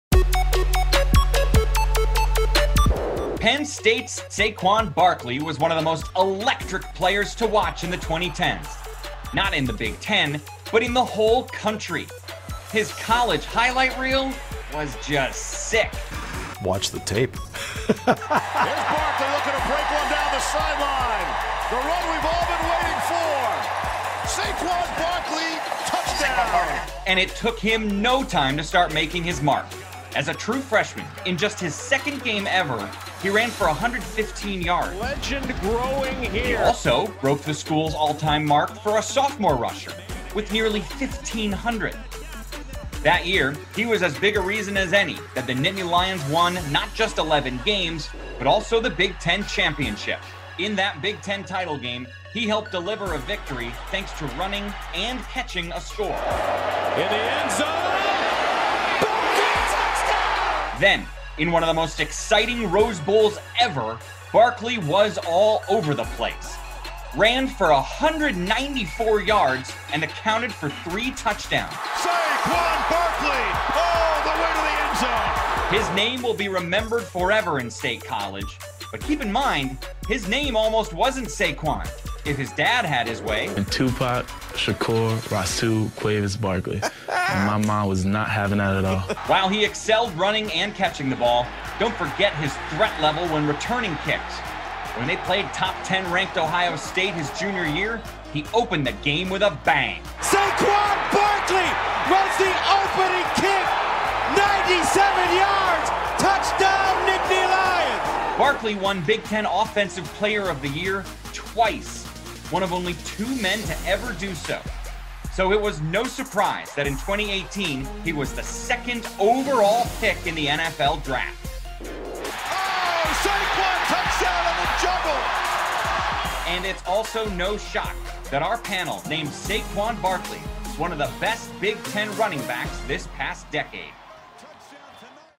BTNAllDecade Team Selection: Penn State RB Saquon Barkley | Big Ten Football
See why Penn State RB Saquon Barkley was tabbed as one of the top Big Ten football players of the past decade.